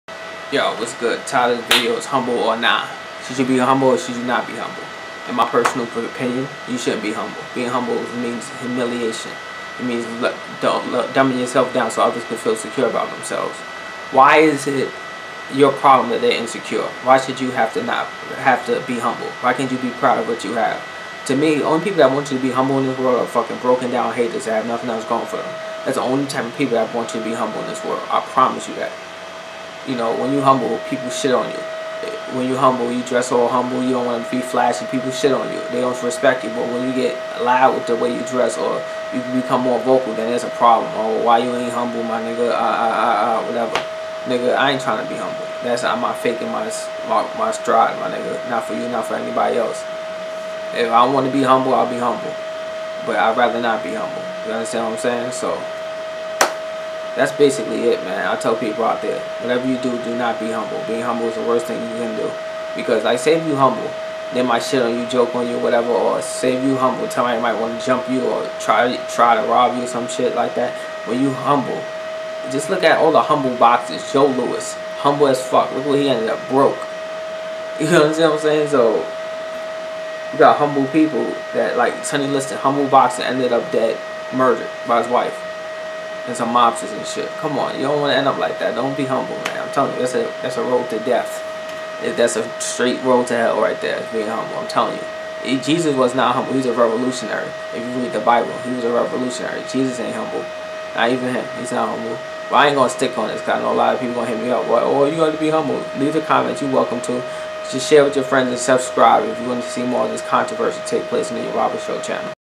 Humble Or Nah? | The Yababa Show™ Vlog #5
Yababa World™ - Where Your Variety is Our Variety
On this episode of The Yababa Show™ Yababa asks the question "humble or nah?"(whether or not someone should be humble).